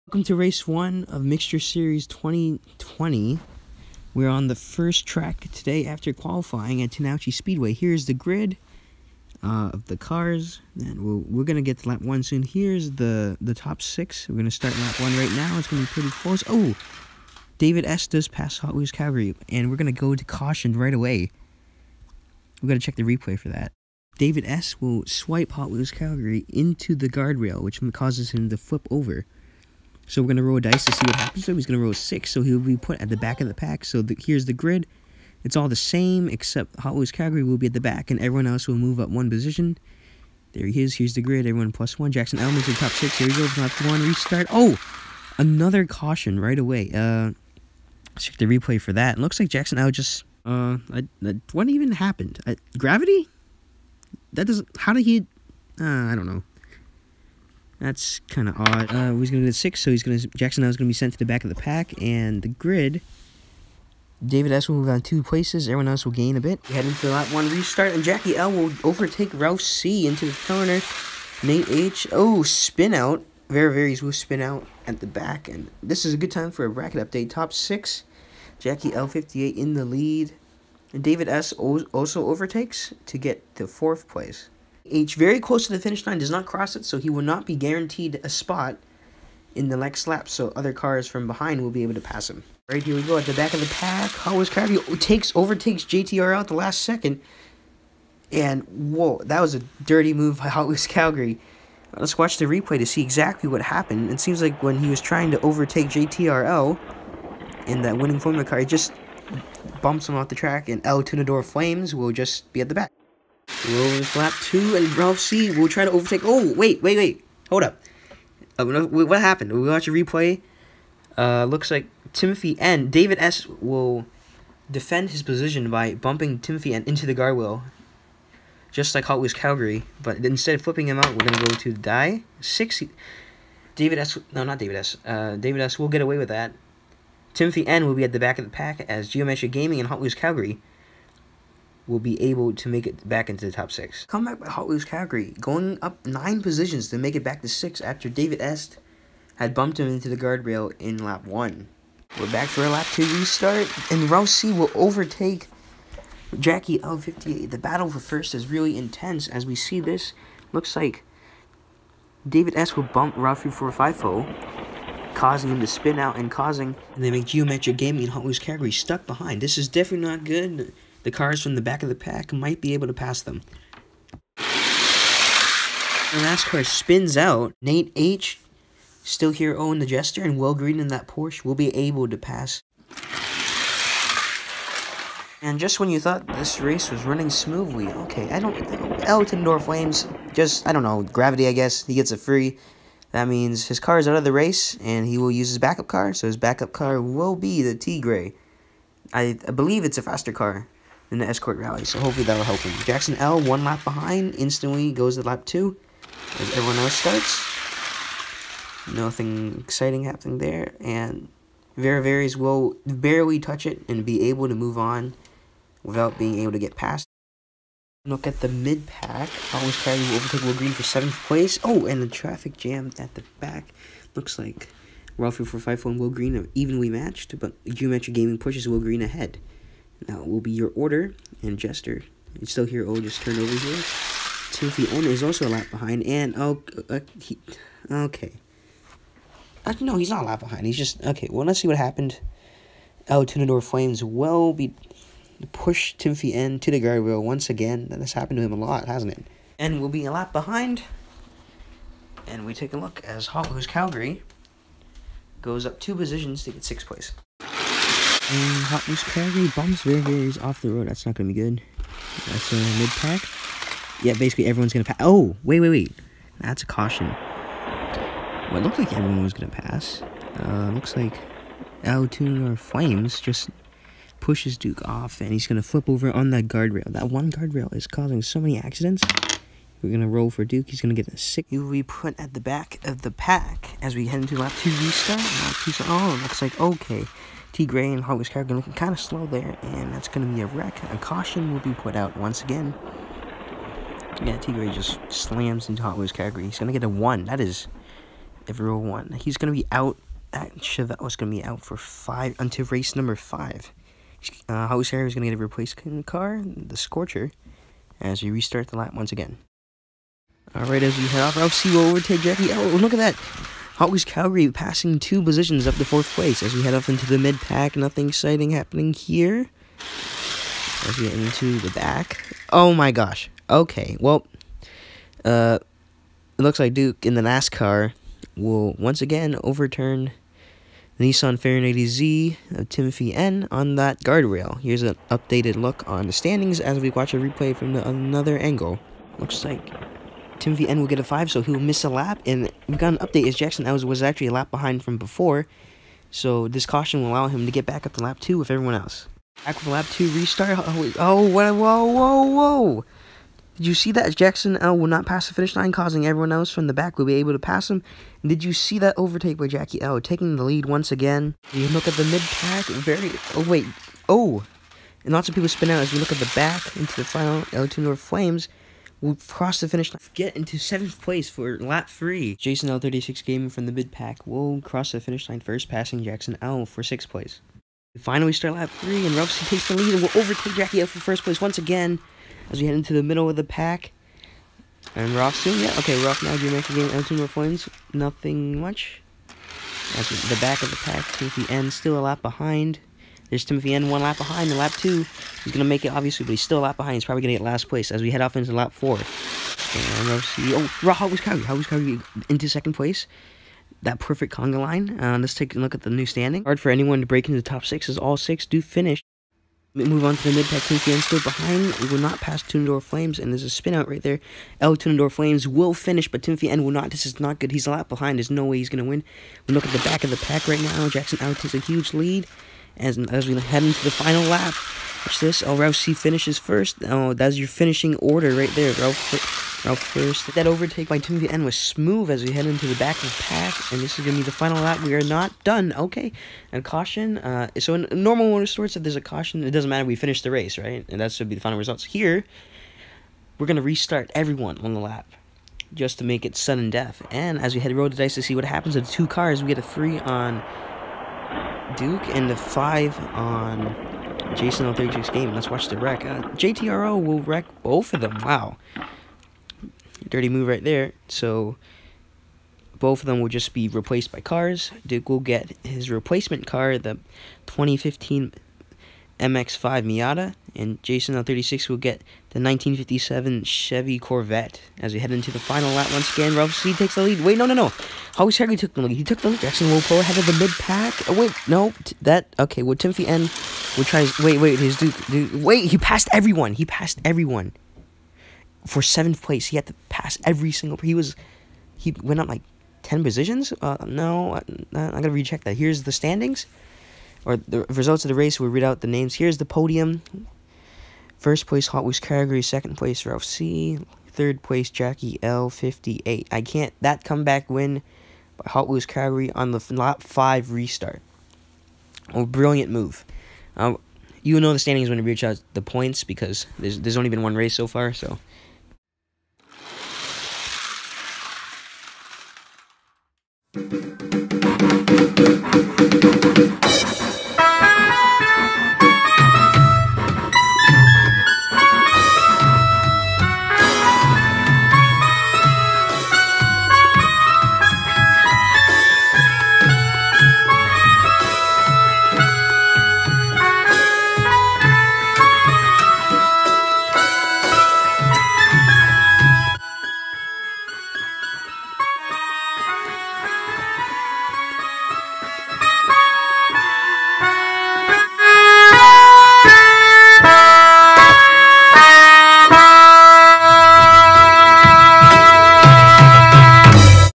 Tinouchy Speedway - Mixture Series 2020 R1
Race one of the Mixture Series 2020, at Tinouchy Speedway. Expect a lot of yellow flags. I`m now starting to see some flaws in my system